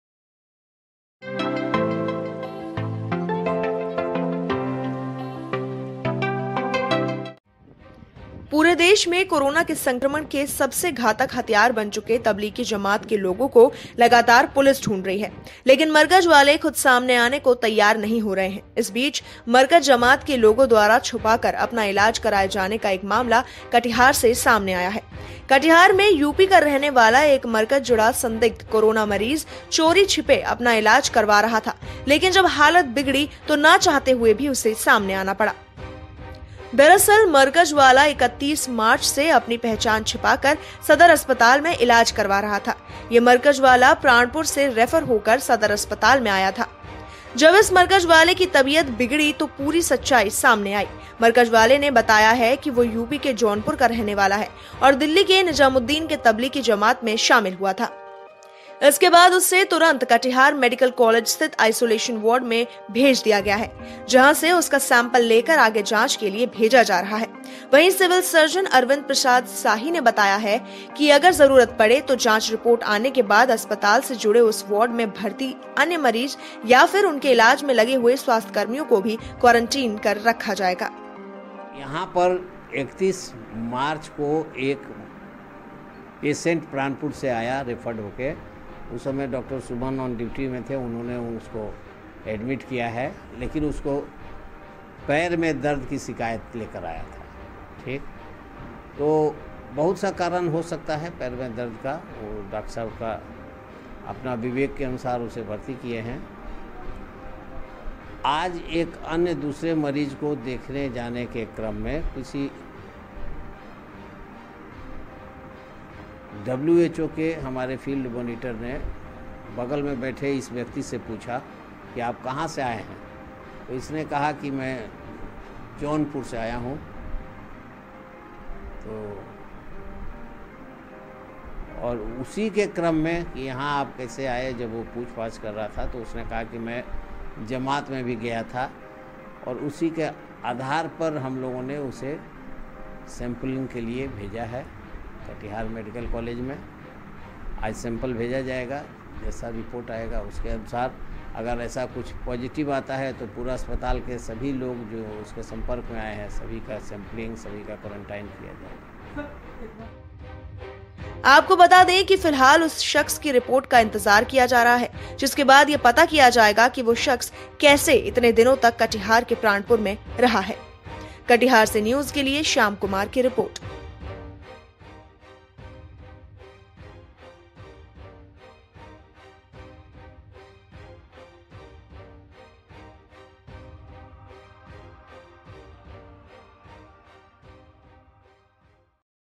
जमात में शामिल हुआ शख्स चोरी-छुप्पे Katihar में करवा रहा था कोरोना का इलाज
पूरे देश में कोरोना के संक्रमण के सबसे घातक हथियार बन चुके तबलीगी जमात के लोगों को लगातार पुलिस ढूंढ रही है. लेकिन मरकज वाले खुद सामने आने के तैयार नहीं हो रहे हैं...